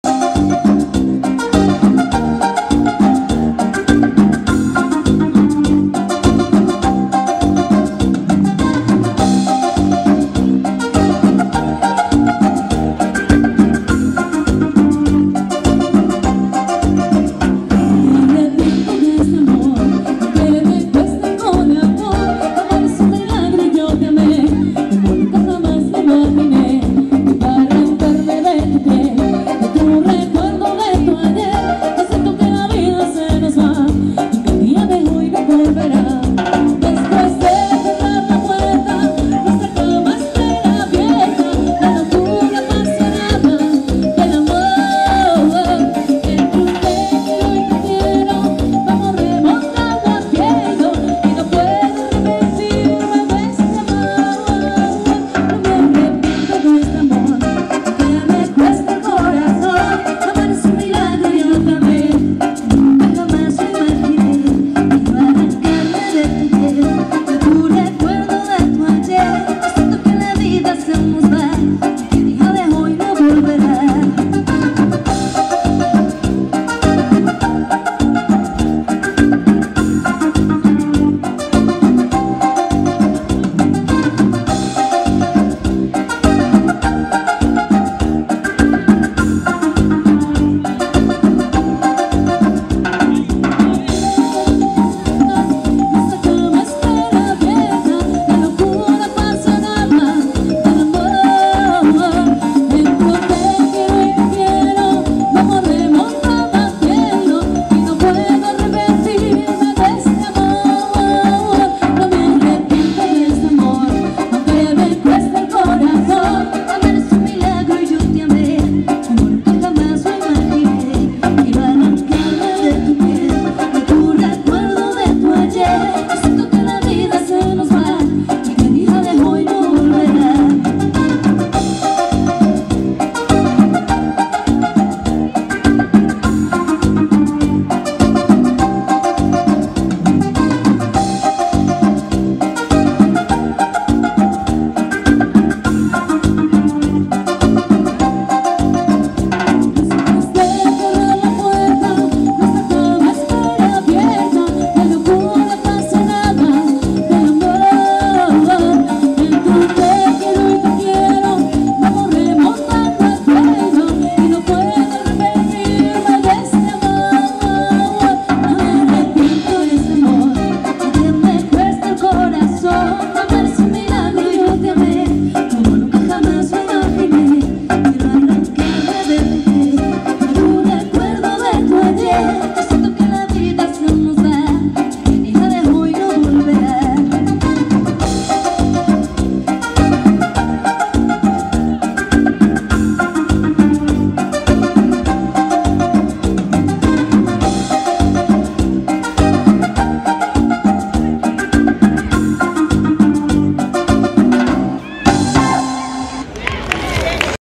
Grupo Alameda de Vigo
Animando a Verbena no día adicado á Patroa (Nosa Señora).